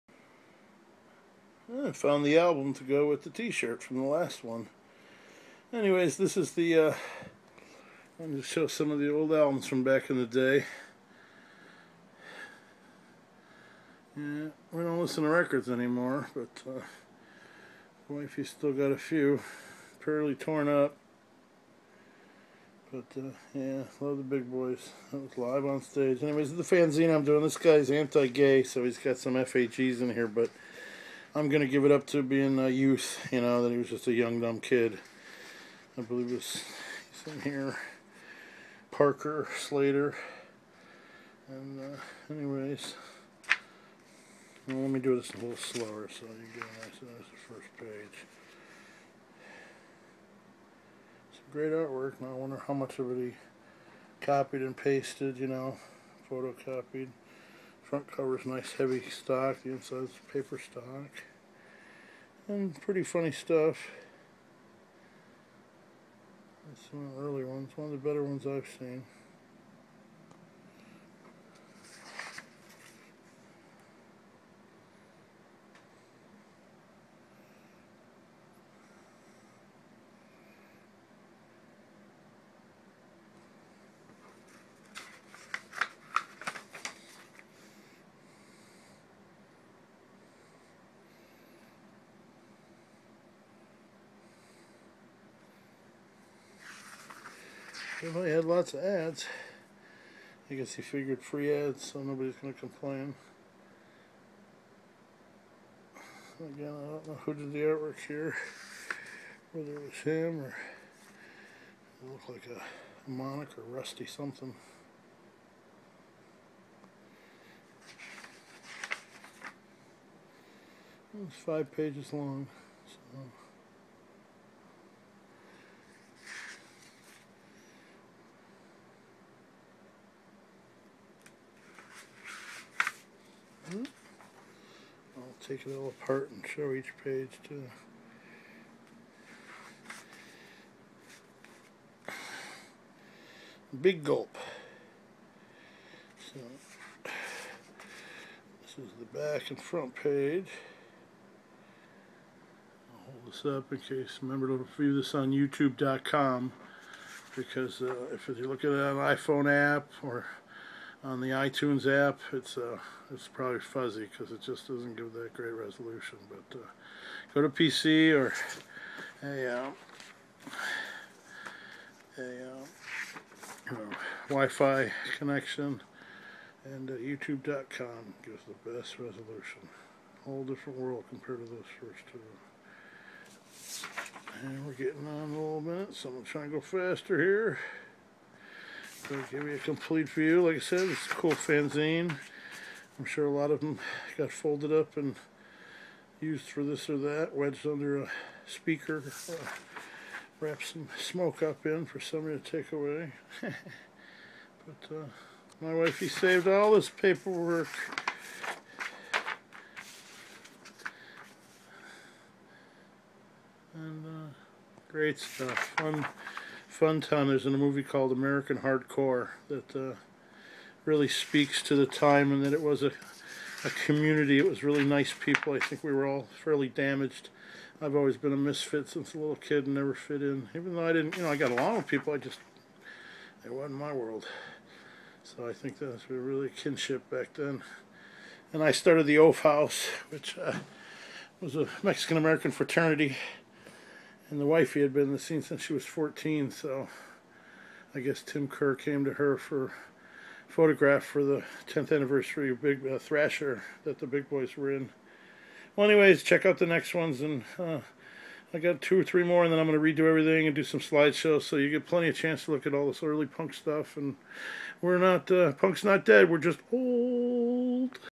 Punk Fanzine "Big Gulp #1" offensive Austin Texas
From Austin 1980's punk fanzine. Had anti- gay offensive slurs. Not my cup of tea, I chalk it to ,stupid youth. Trying hard to be offensive. Please check out first 5 videos, with more Show fliers, fanzines and posters. My wife Dana save most of these. ( not for sale, just sharing)